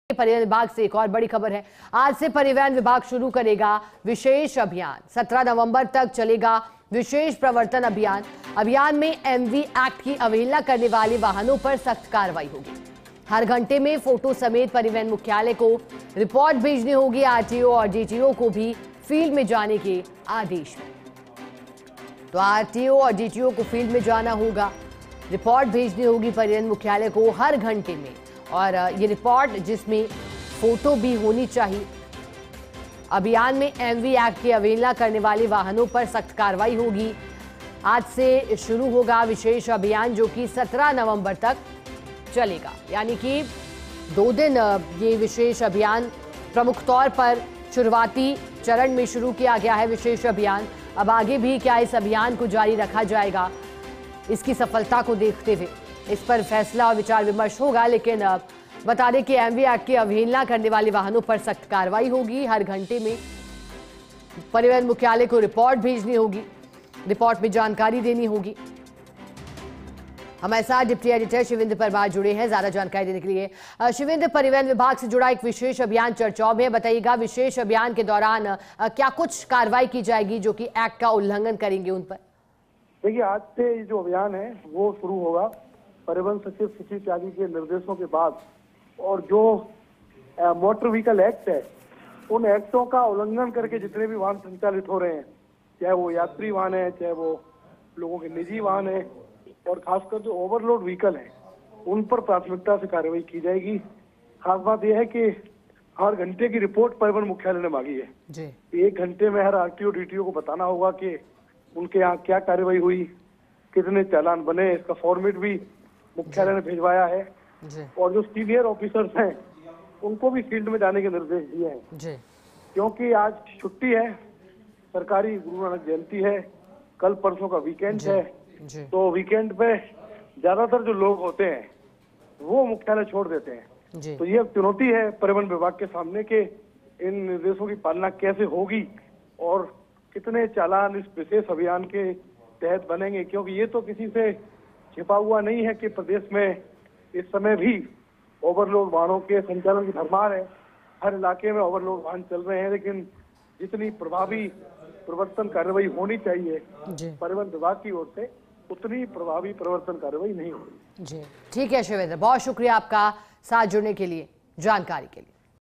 आज से परिवहन विभाग का विशेष अभियान, MV Act की अवहेलना पर होगी सख्त कार्रवाई | Transport Department
About this Video:
आज से परिवहन विभाग का विशेष अभियान, MV Act की अवहेलना पर होगी सख्त कार्रवाई  | Transport Department | 15 November 2024